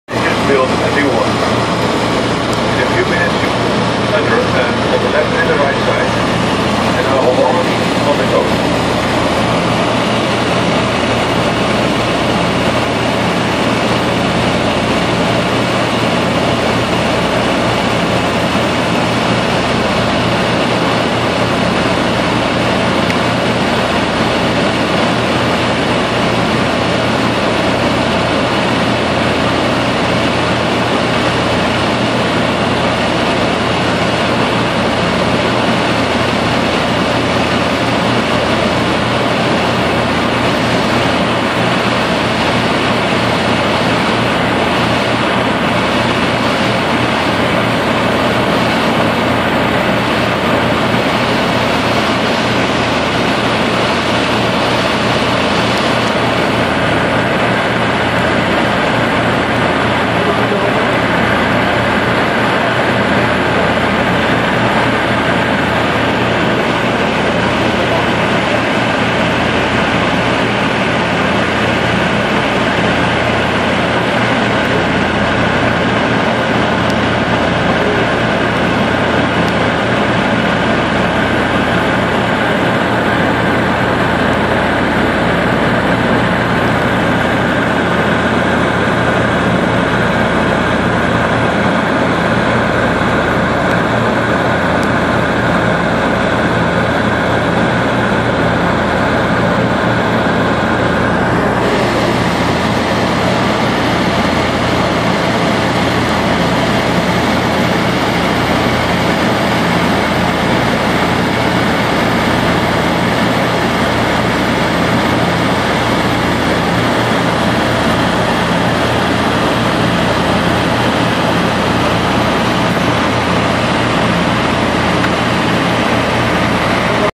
Flight over Arromanches Dc3 (PH-PBA)
Survol des batteries de Longues et d'Arromanches en  Dc3 lors des Daks over normandy le 7 juin 2014